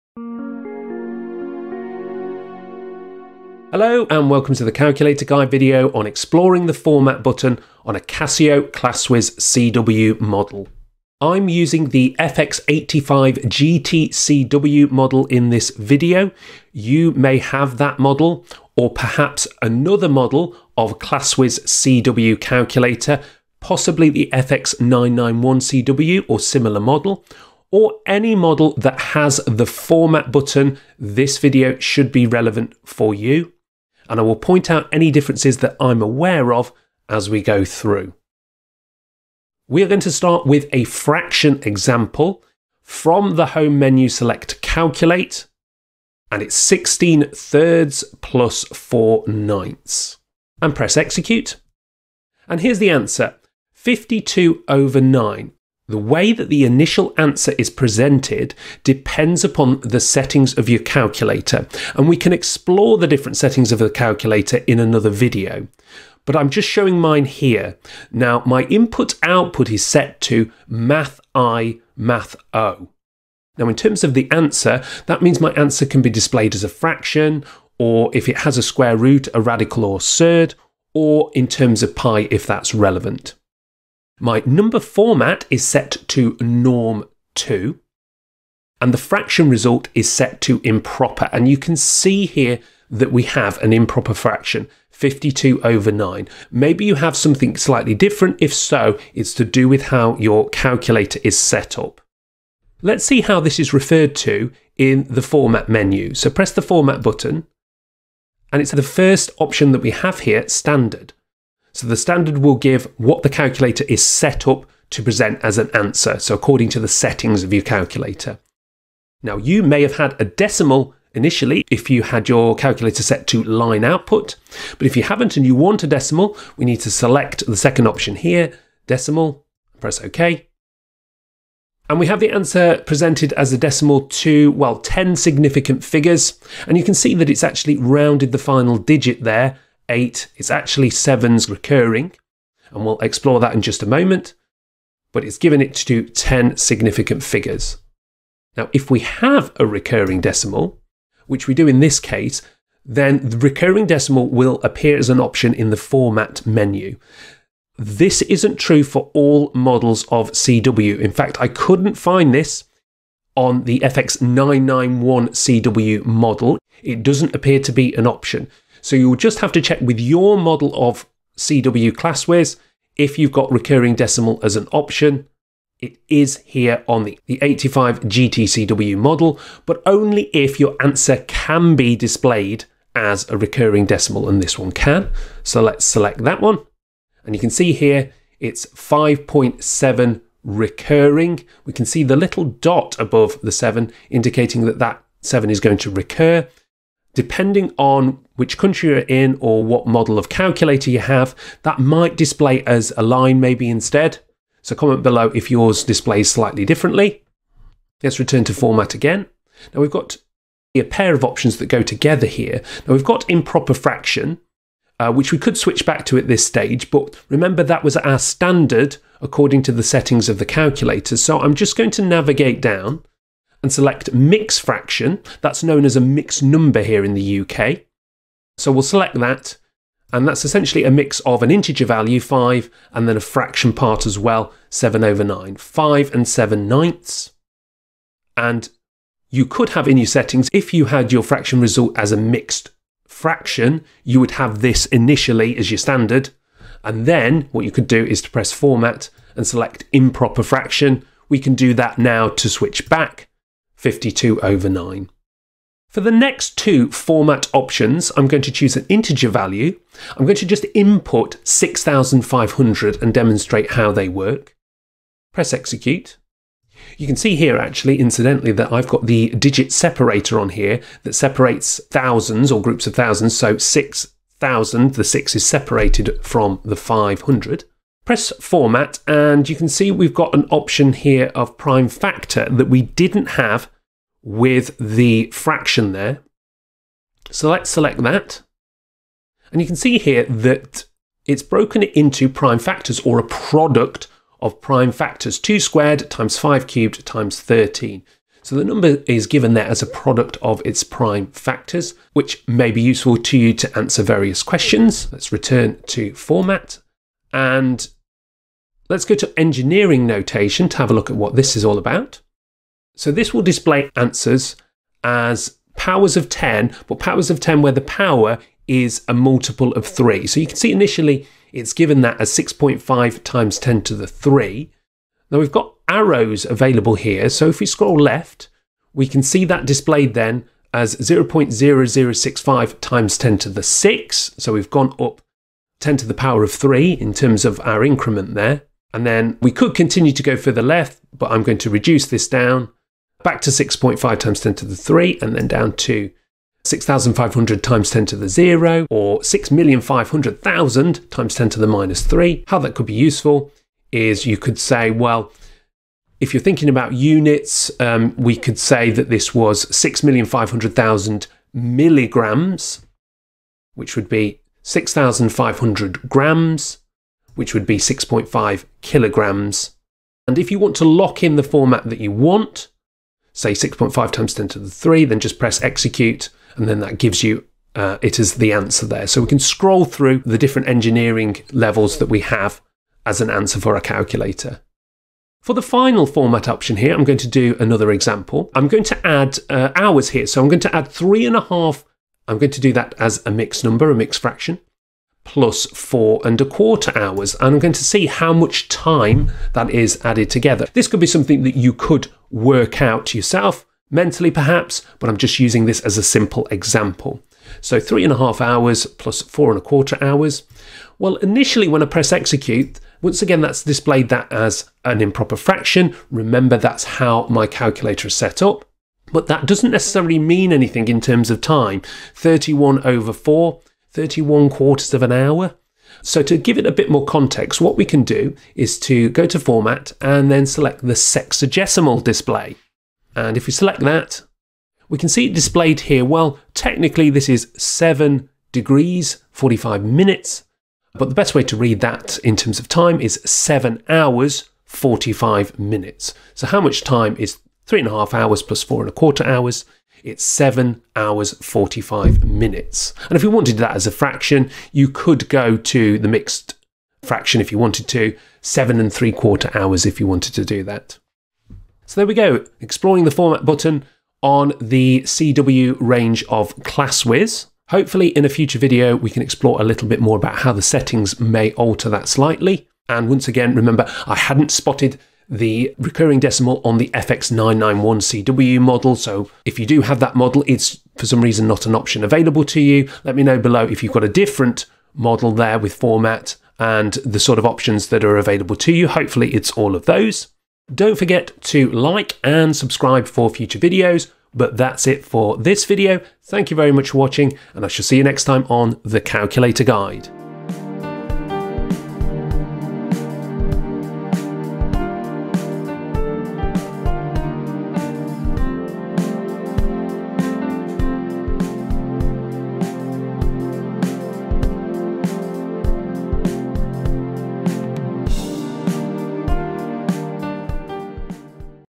Exploring The Format Button On A Casio Classwiz CW Model
⨸ Gᴇᴛ Aɴ fx-991CW Nᴏᴡ ➜ ⨸ Aᴍᴀᴢᴏɴ Sʜᴏᴘ ➜

In this video we look at all the options of how to display your answer using the Format Button. This includes changing between a fraction, decimal and mixed number as well as prime number de-factorisation, Engineering notation and Sexagesimal display for time calculations.